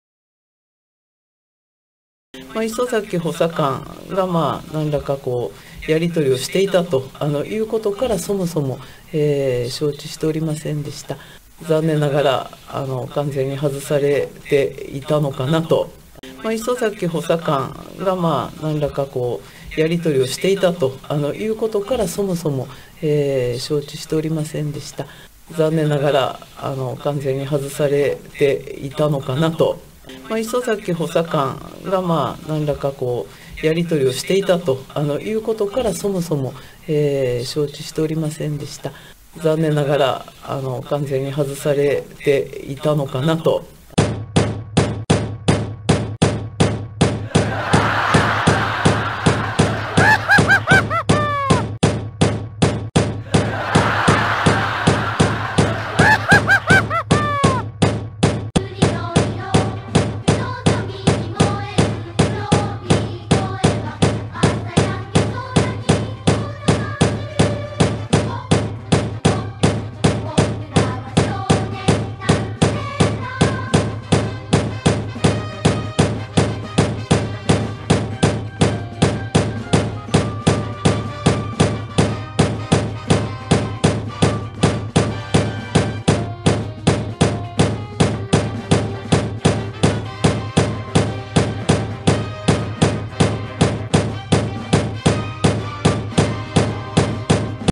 高市嘘ばらまきを総務省完全認定により、高市は逮捕か、議員クビか？
23/3/22　ＷＢＣ優勝の日に、高市犯罪が明白に。